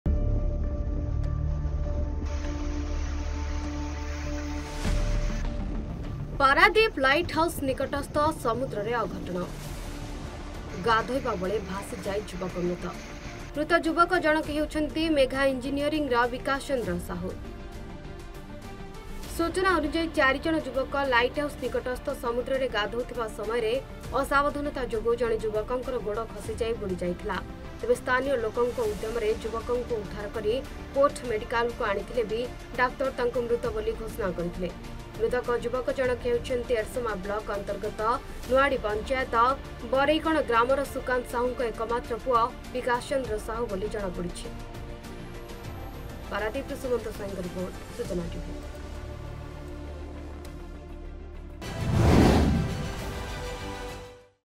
ଗାଧୋଇବା ବେଳେ ଭାସିଯାଇ ଯୁବକ ମୃତ
ପାରାଦୀପ ଲାଇଟ୍ ହାଉସ ନିକଟସ୍ଥ ସମୁଦ୍ରରେ ଅଘଟଣ। ଗାଧୋଇବା ବେଳେ ଭାସିଯାଇ ଯୁବକ ମୃତ। ମୃତକ ଯୁବକ ଜଣଙ୍କ ହେଉଛନ୍ତି ଏରସମା ବ୍ଲକ ଅନ୍ତର୍ଗତ ନୁହାଡ଼ି ପଞ୍ଚାୟତ ବରେଇକଣ ଗ୍ରାମର ସୁକାନ୍ତ ସାହୁ ଙ୍କ ଏକମାତ୍ର ପୁତ୍ର ବିକାଶ ଚନ୍ଦ୍ର ସାହୁ ବୋଲି ଜଣାପଡିଛି।

Dear Viewers,